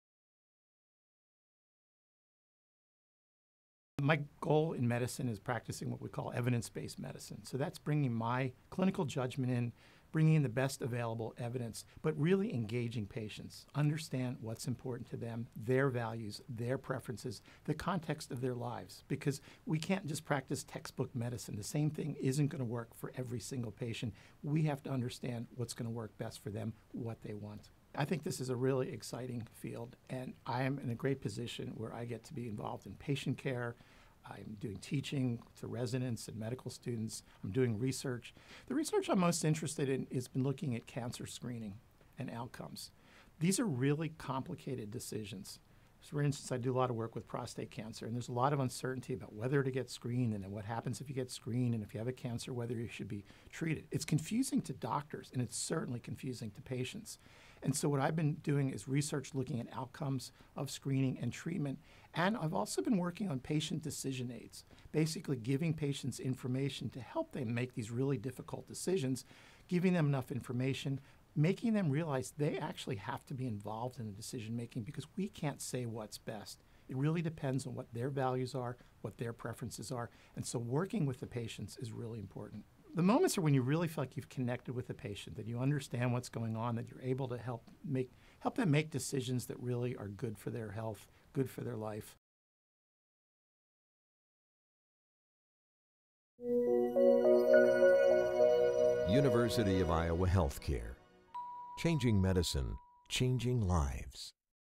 Richard Hoffman, MD, MPH
Richard Hoffman is a general internal medicine physician at the University of Iowa Hospitals and Clinics, Iowa City, Iowa.